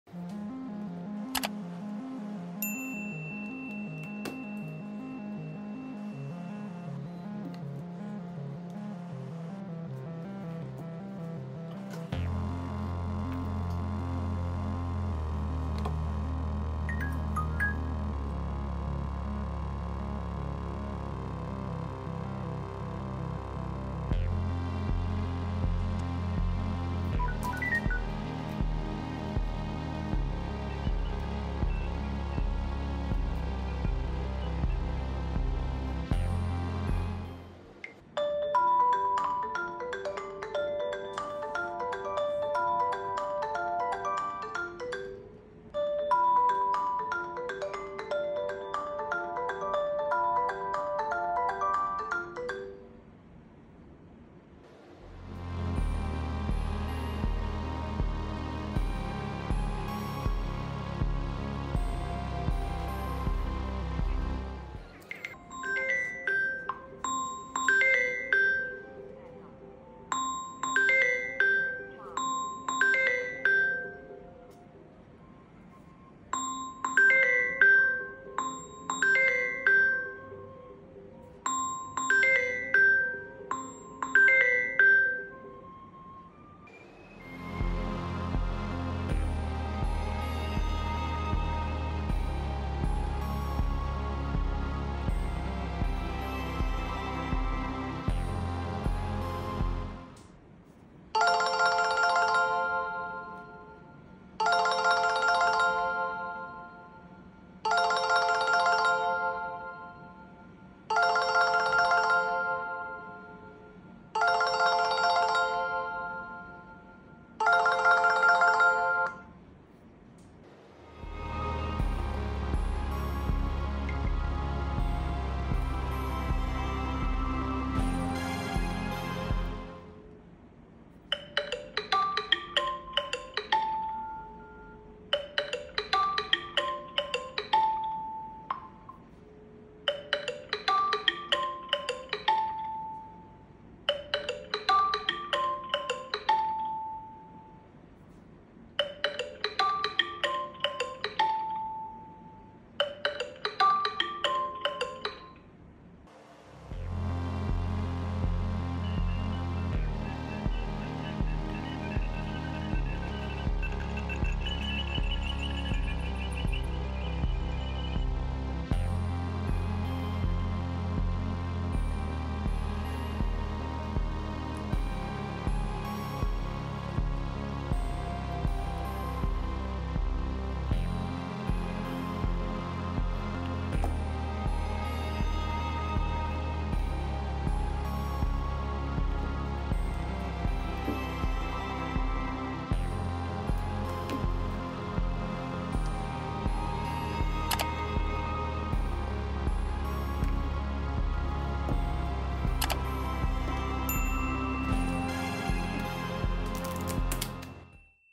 BootAnimation Nokia G31 + Samsung A14 + BlackBerry + Xiaomi RN12 + OPPO A54 Incoming & Outgoing Call
Boot Animation Nokia G31 + Samsung Galaxy A14 + BlackBerry Passport + Xiaiomi Redmi Note 12 + OPPO A54 Incoming & Outgoing Call